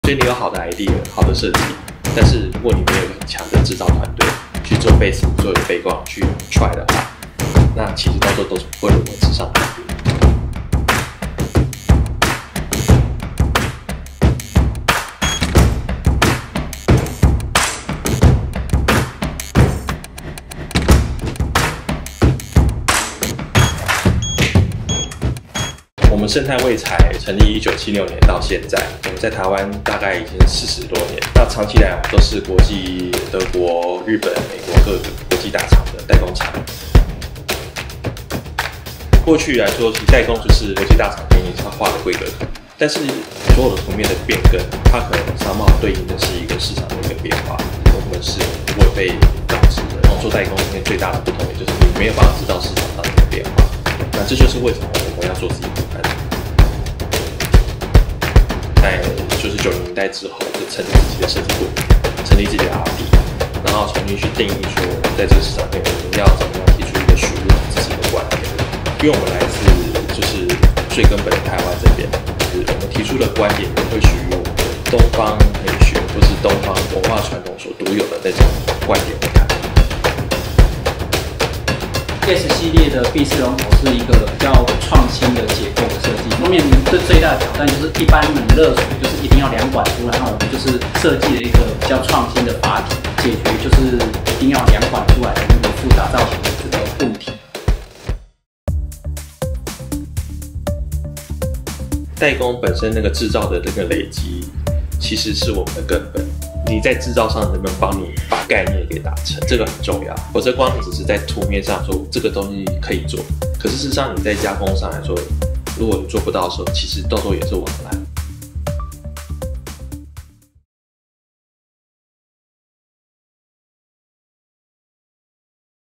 設計製造研究所｜傳產代工如何進軍品牌？勝泰衛材 3 關鍵創造市場價值
勝泰衛材成立設計部門、研發部門，創立自有品牌「Justime 巧時代」品牌、建立自有通路，大力行銷台灣水五金，改變 OEM 和 ODM 生產方式，品牌在全球已有 28 個代理商，究竟它們在國際發光的關鍵因素是什麼？